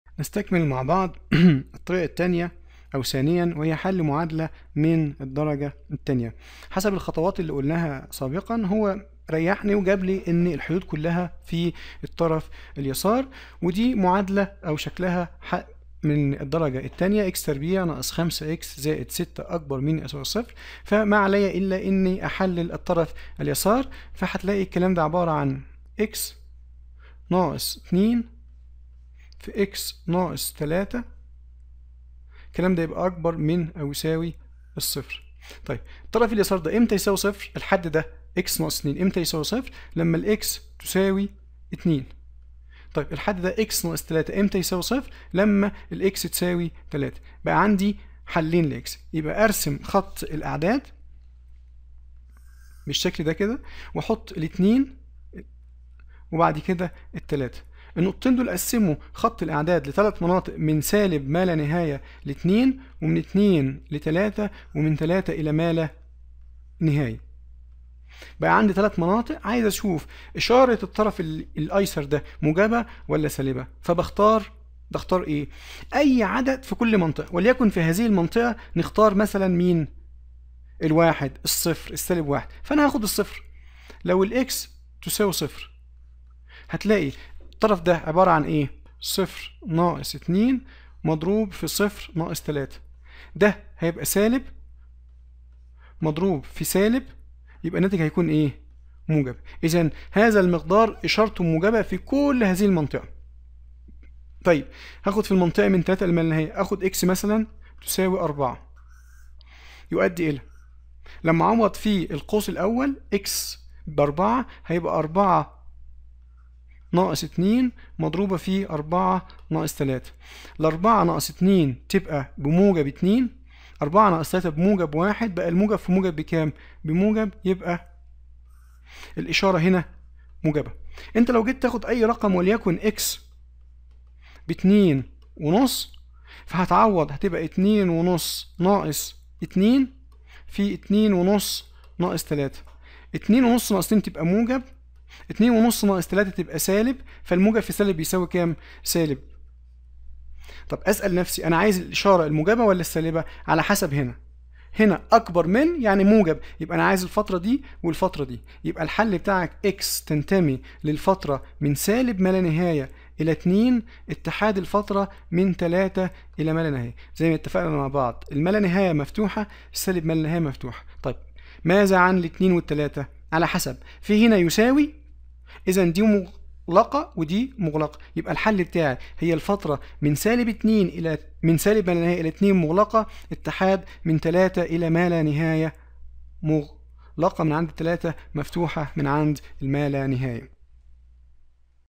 حساب التفاضل والتكامل - الوحدة 1 : المتباينات - 5 - Inequale
المقررات المفتوحة
الوحدة الأولى - مراجعة لما سوف نحتاجه قبل التفاضل والتكامل
مثال علي حل متباينة من الدرجة الثانية في متغير واحد.